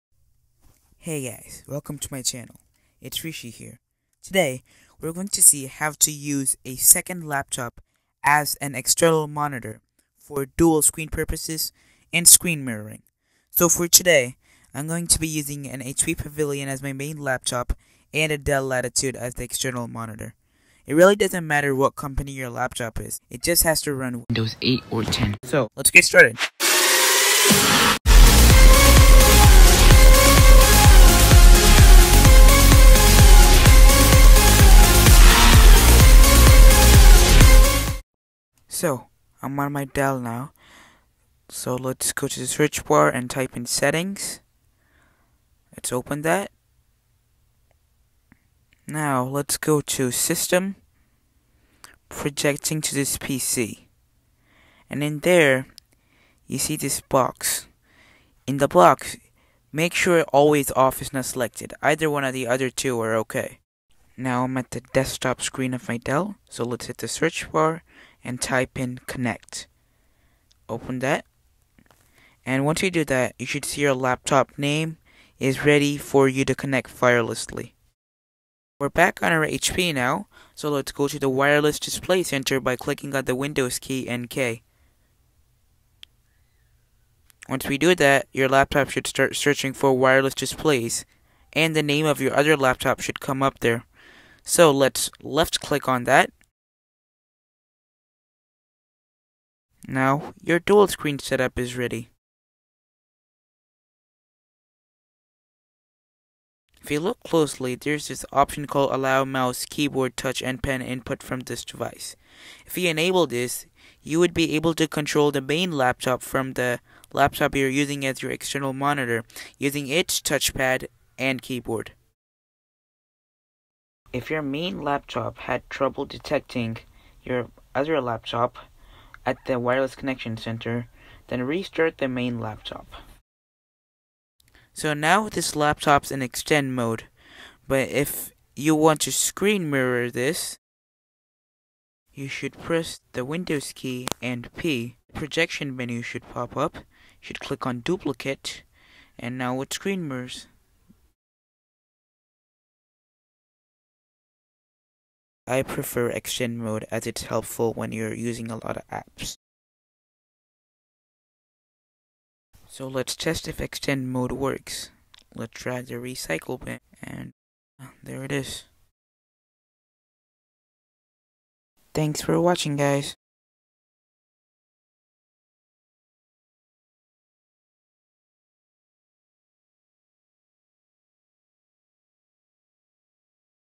How to use your Old Laptop as a Second Monitor
Are you tired using multiple apps on one single Windows laptop and you have a older laptop lying around? Or you just want to screen mirror one laptop to another. Well this video is the solution for you. It doesn’t matter which brand your laptop is, it just has to run Windows